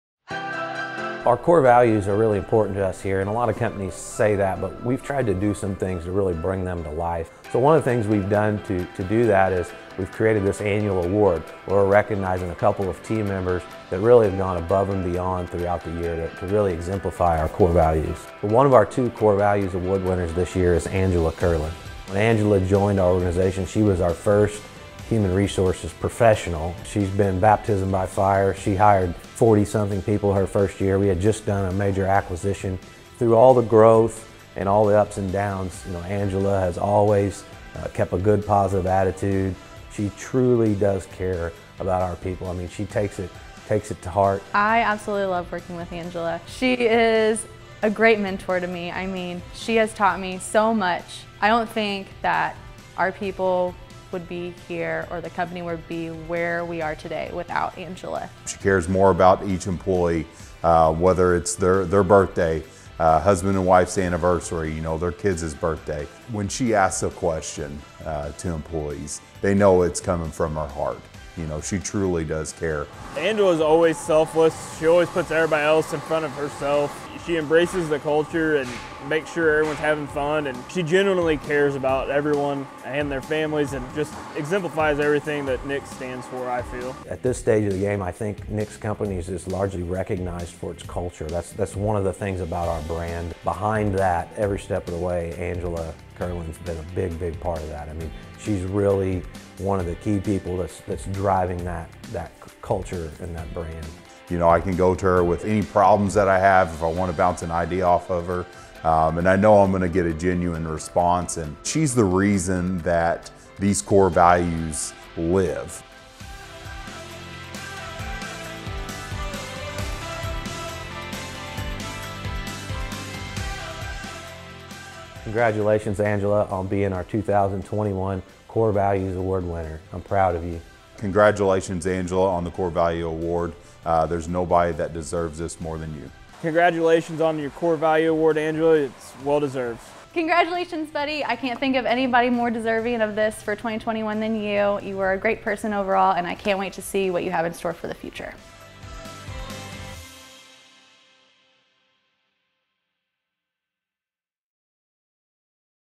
Core Values Award 2021 | Angela Kirlin
Each year we give out two Core Values Awards.  With how important our core values are to our company, this award was created to celebrate team members who best exemplify our core values throughout the year.

We share one of our Core Values Award winner last week and we're excited to share the second one today.  Our second Core Values Award winner for 2021 is Angela Kirlin, our HR Manager.  Angela is a driving force behind our culture and our core values.  You'll often see her helping out the local community while also being very engaged with each of our team members.

Watch this short video to hear about how Angela truly cares about each of our team members and let's congratulate Angela on this well-deserved award!